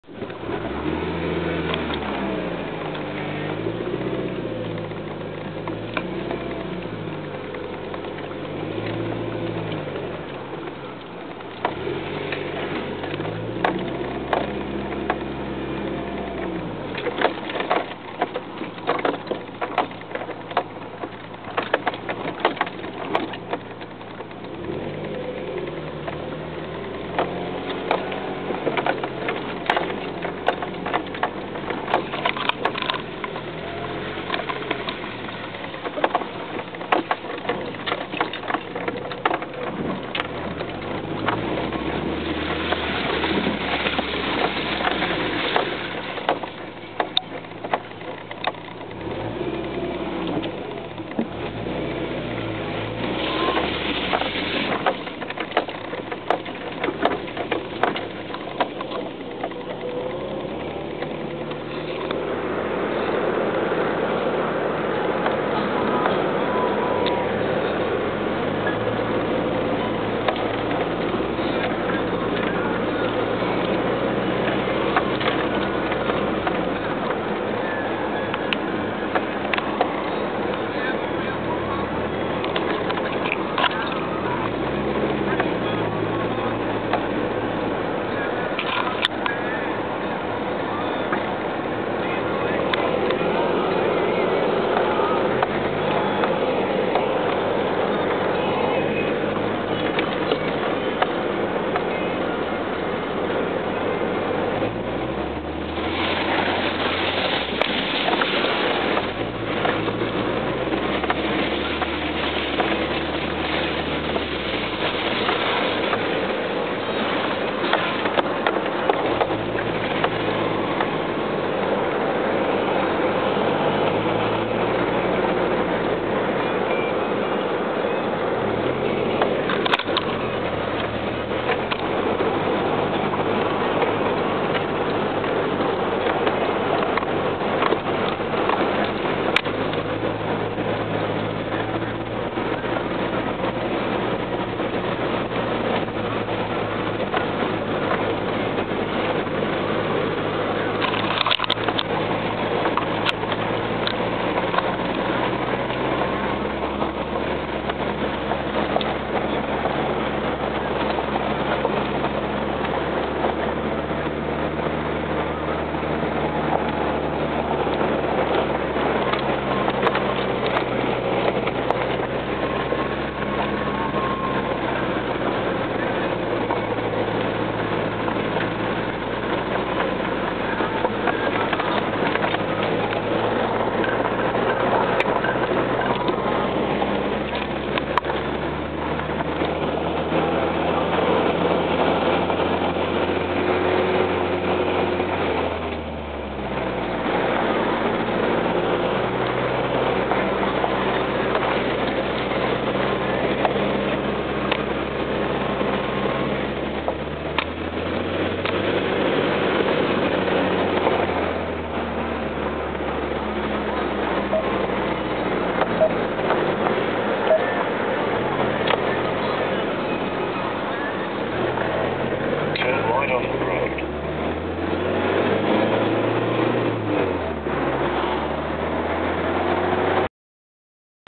Farmington River access road
I wasn't sure my (rear-wheel-drive) truck was going to make it through the mud. Unfortunately for you, it did...so this video is BORING.